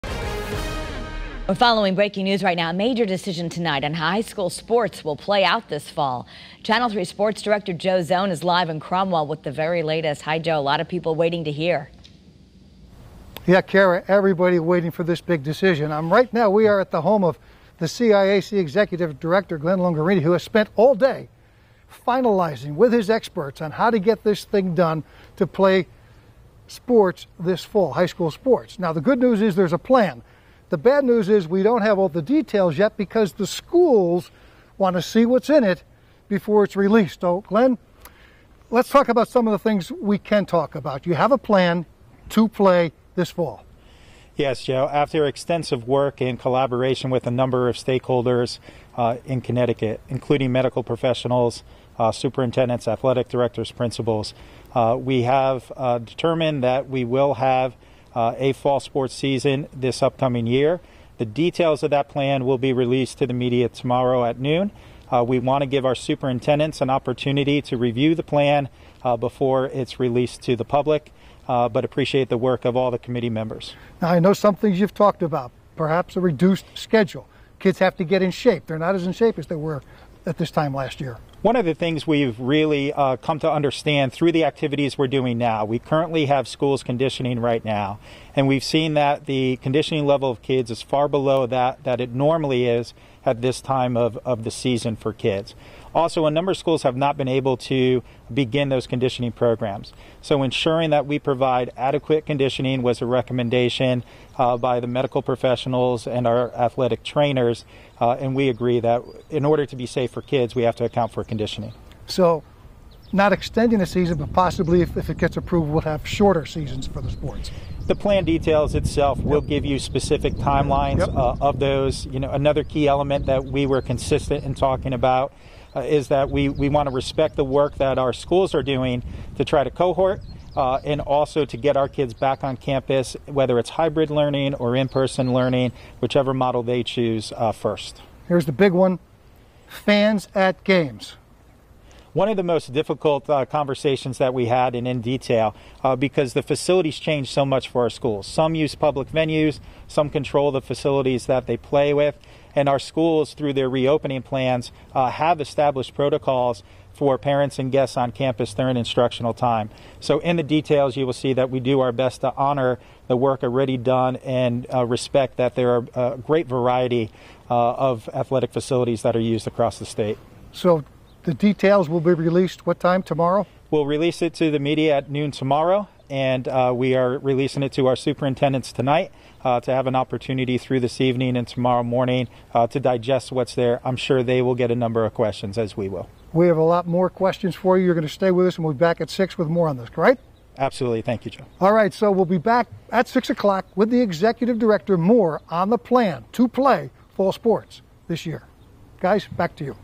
VIDEO: CIAC announces fall sports plan set to be released on Friday
The CIAC has determined there will be a fall sports season this year and expect to release the full plan on Friday.